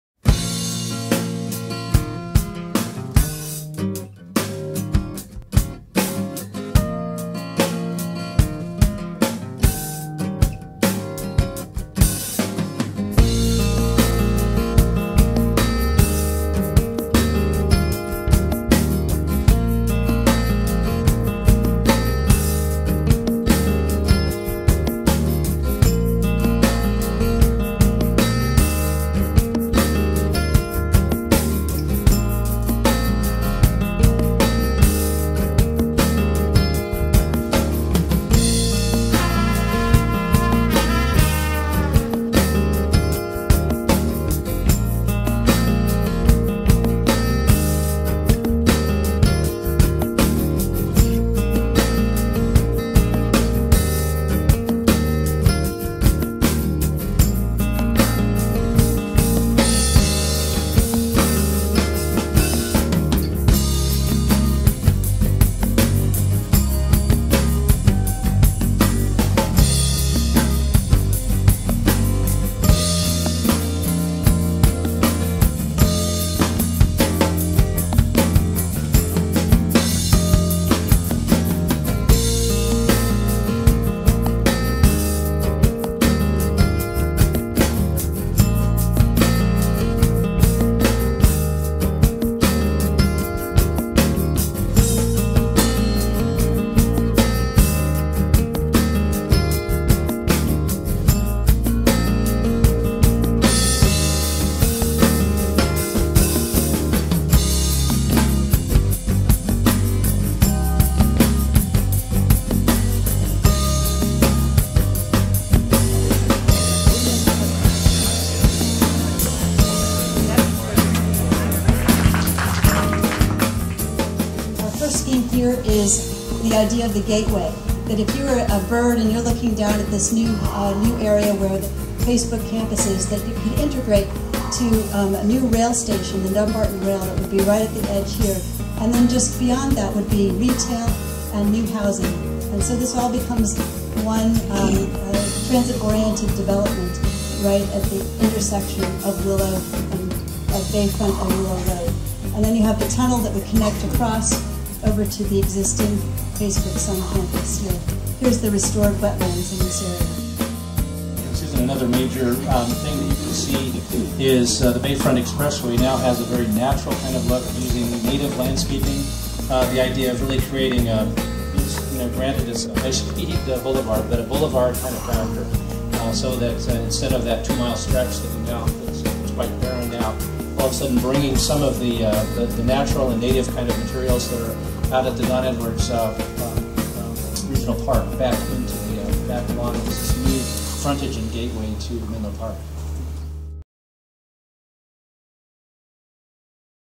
Facebook Design Charrette
A look into the creative process as DES Architects participates in a design charrette for the Facebook campus in Menlo Park California.

A  short subject film by Rick Garcia.